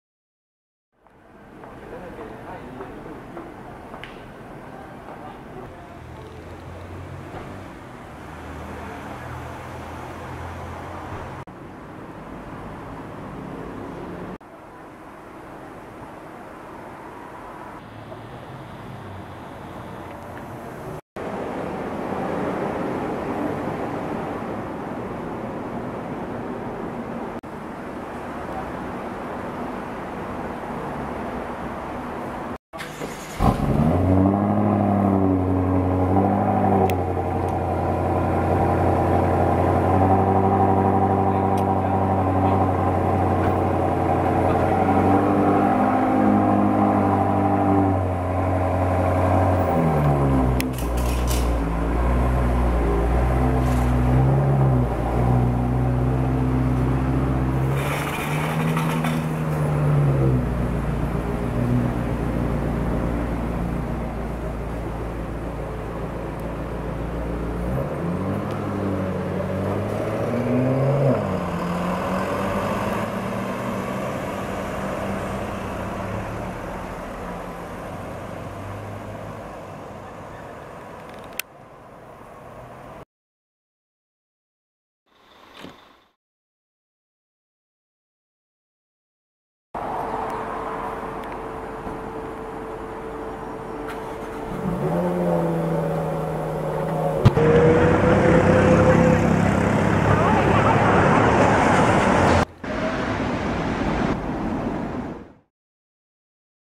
Akrapovic BMW M5 F10 Starts up!
We film a nicely specced BMW M5 F10 starting up and leaving St Regis! The car comes with the optional clear rear lights, and a cheeky little carbon rear spoiler, and looks ace!